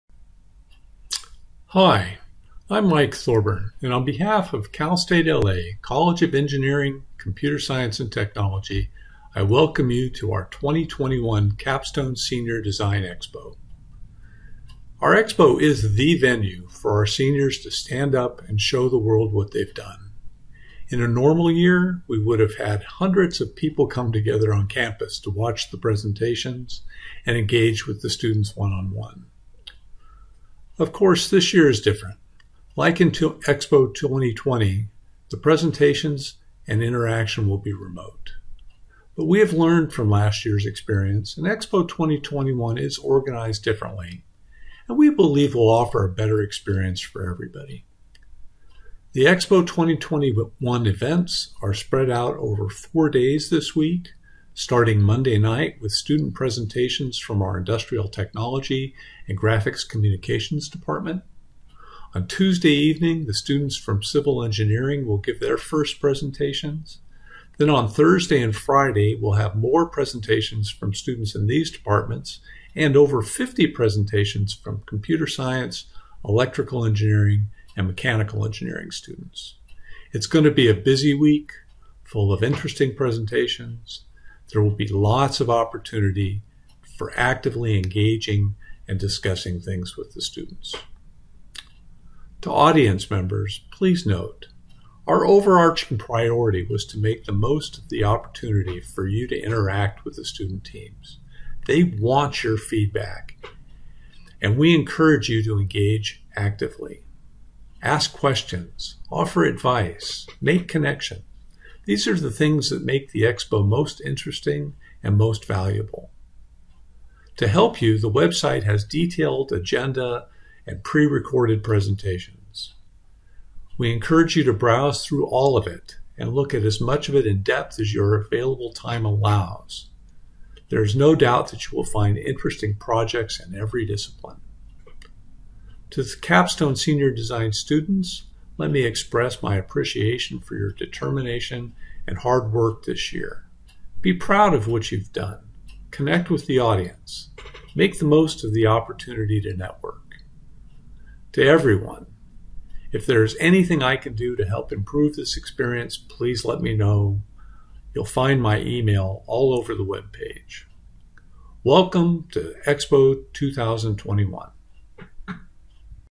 ECST Capstone EXPO 2021 Welcome
Welcome to Cal State LA's College of Engineering, Computer Science, and Technology's Capstone Senior Design program's Expo 2021.

Message by, Dr. Michael Thorburn, ECST Capstone Director.